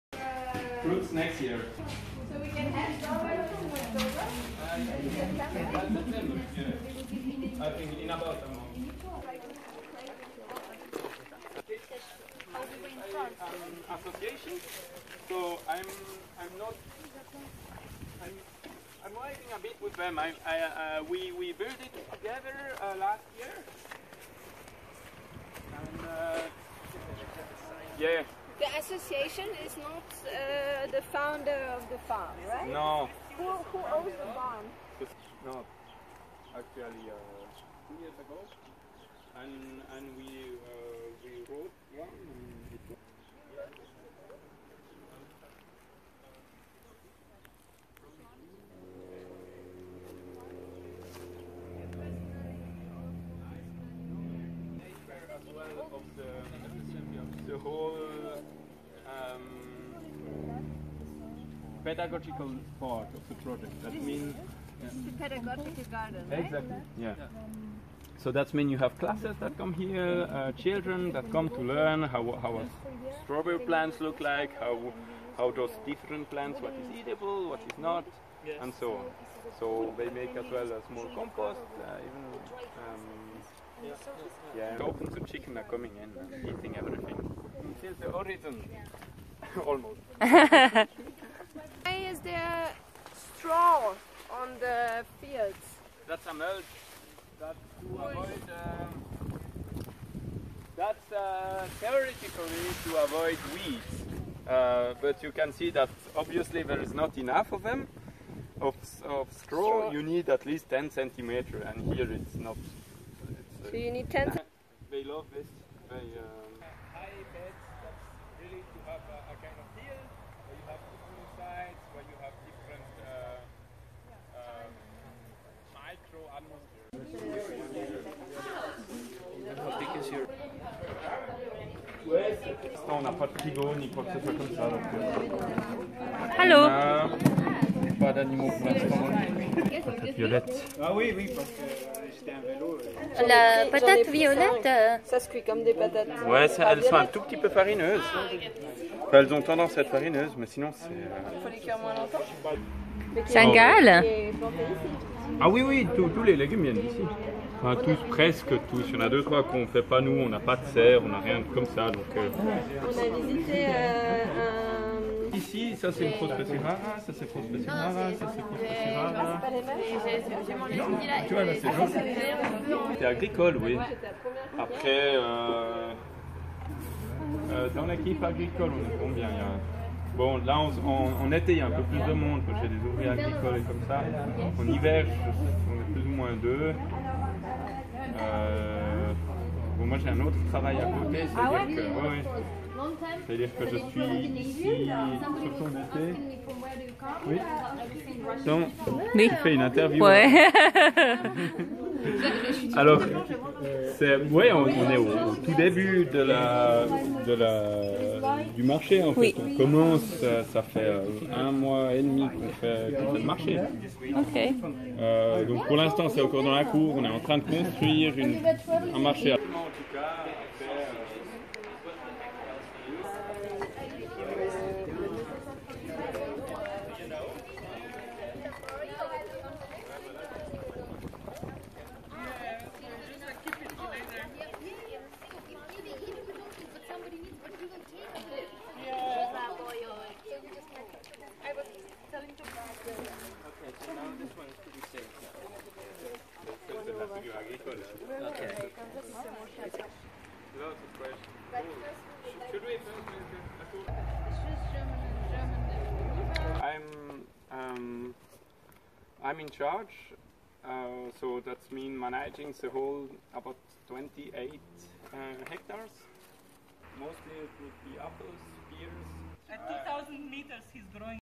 fairpractice Pedagogical garden at Rovéréaz agro - ecological farm
On our faircrawl in Lausanne we visited this wonderful fun where we learned a lot about local vegetable species and plants.
A real fairspot :)